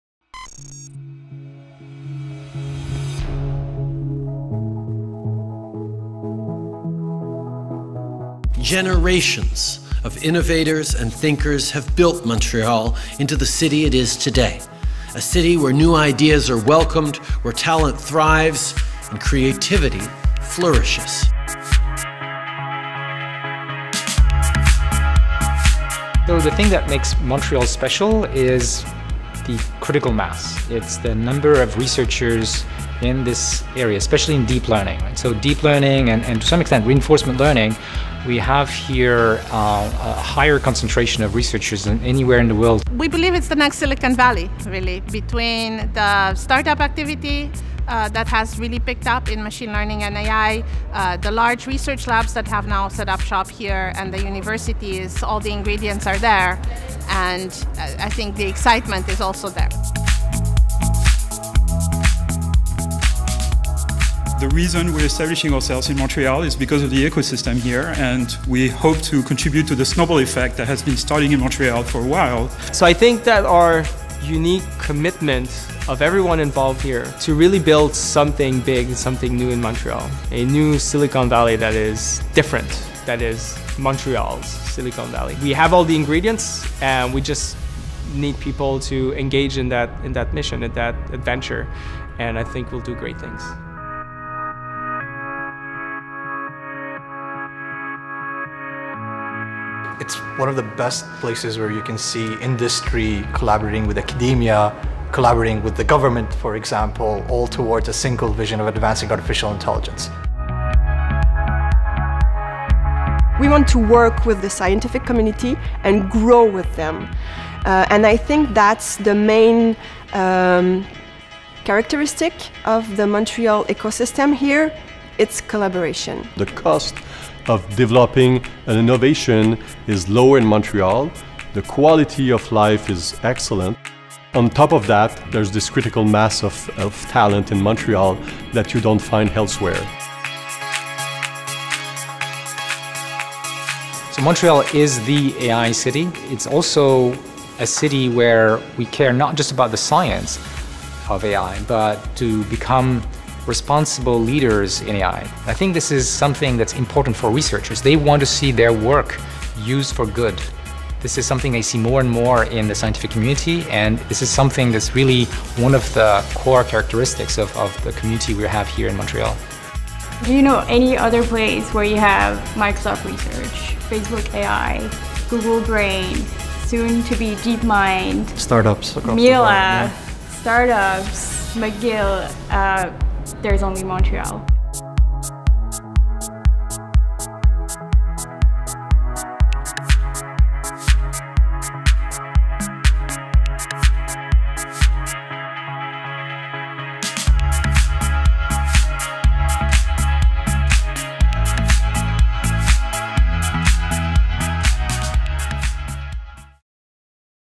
Artificial Intelligence Montreal Powerhouse
AITechMTL , Discover Montréal's AI ML datascience scene!

A seductive overview of the thriving Montréal's AI ecosystem - starring world famous AI researchers and the most innovating companies in deep learning, data science and machine learning such as :
- Google Brain
- DeepMind
- Maluuba
- Element AI
- Samsung
- Thales
-  Imagia

Key speakers:
- (0:08) Justin Trudeau, Prime Minister of Canada

- (0:26) Yoshua Bengio - Full Professor, Université de Montréal & Head of the Montreal Institute for Learning Algorithms (MILA)

- (0:43) Doina Precup - Associate Professor- McGill School of Computer Science - DeepMind

- (01:04) Yann LeCun - Director of AI Research - Facebook

- (01:13) Hugo Larochelle - Lead of Google Brain team, Montréal

- (1:40) Mohamed Musbah - Head of Product - Microsoft Research Maluuba, Montréal

- (1:52) Valérie Bécaert- Director Reasearch group - Element AI

- (2:06) Alexandre Le Bouthiller- Founder & COO - Imagia

Video produced by TCB MÉDIAS